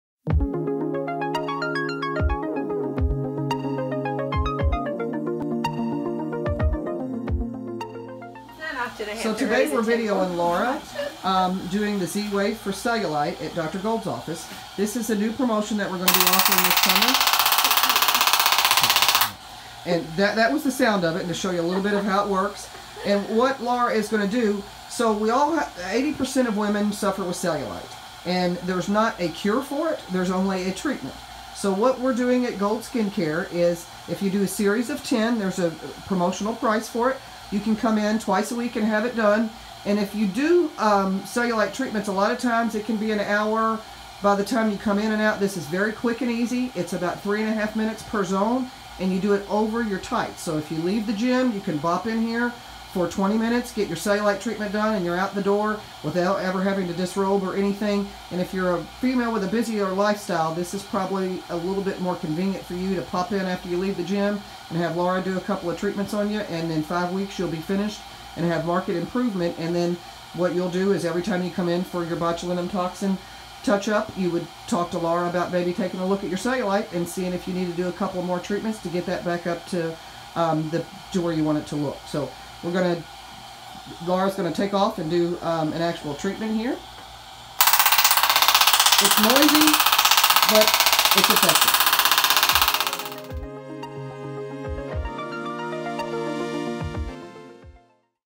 Gold Skin Care Center Offering Speedy, Effective Z Wave Cellulite Treatments over Gym Pants
Gold Skin Care Center demonstrated a quick and comfortable Z Wave treatment for the reduction of the appearance of cellulite.  In this video, Z Wave specialist, Laura, showed how the treatment is performed using the Zimmer MedizinSystems Z Wave.  Remarkably convenient for busy patients, the procedure can be done over light pants like gym leggings.